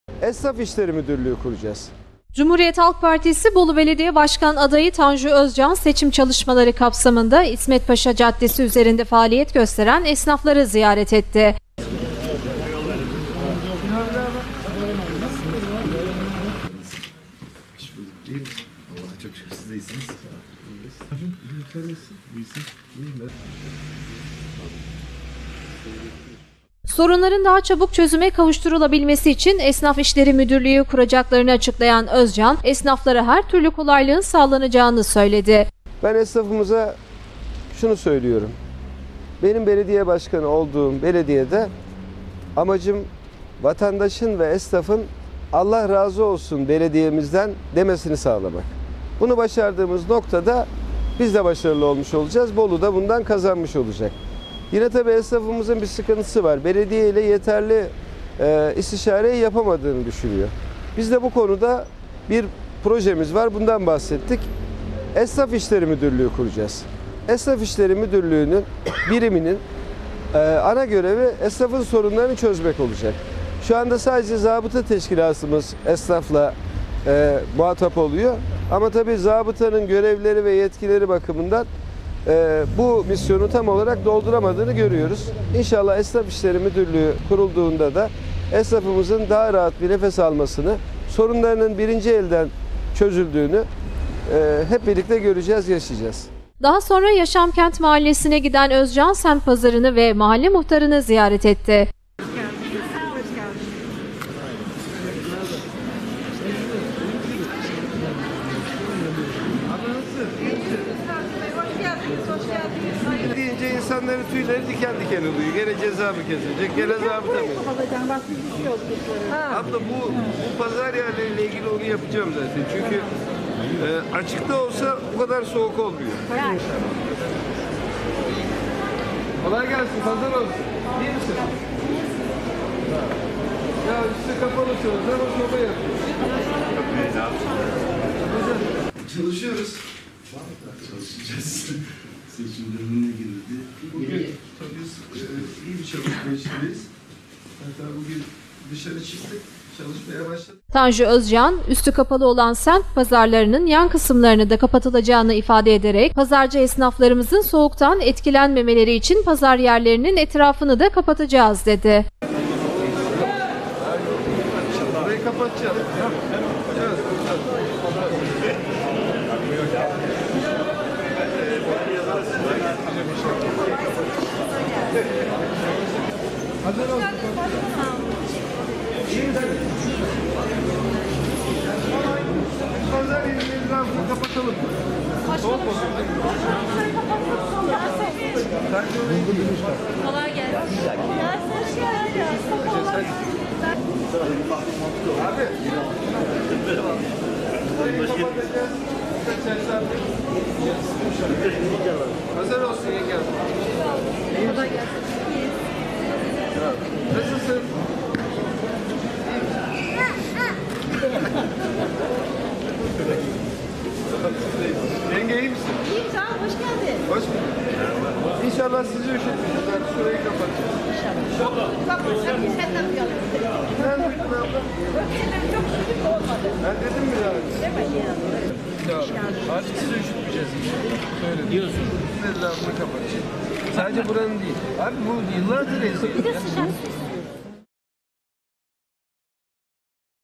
TANJU ÖZCAN “ESNAF İŞLERİ MÜDÜRLÜĞÜ KURACAĞIZ” (04.01.2019)
Cumhuriyet Halk Partisi Bolu Belediye Başkan Adayı Tanju Özcan, seçim çalışmaları kapsamında İsmetpaşa Caddesi üzerinde faaliyet gösteren esnafları ziyaret etti.